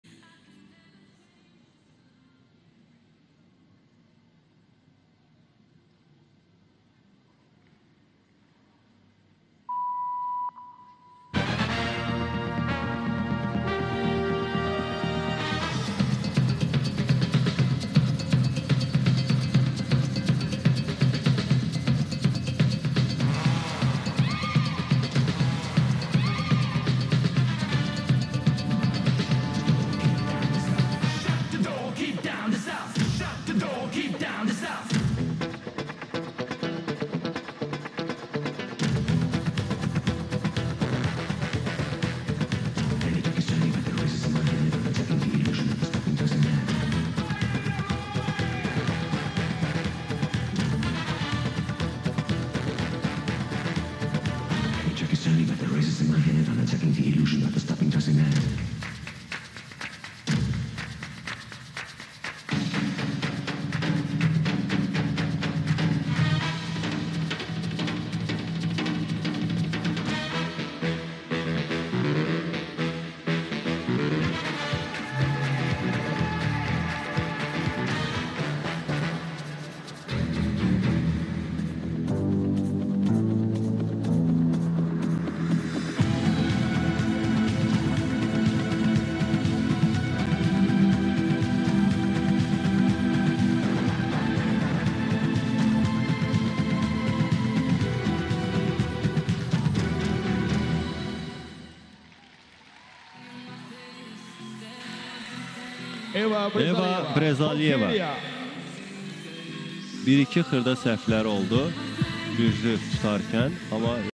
Eva Brezalieva (BUL) clubs final / World Cup Baku 2023
Score 31.650
Sorry for the quality, my go-to TV did not stream this WC):
All rights to music owners.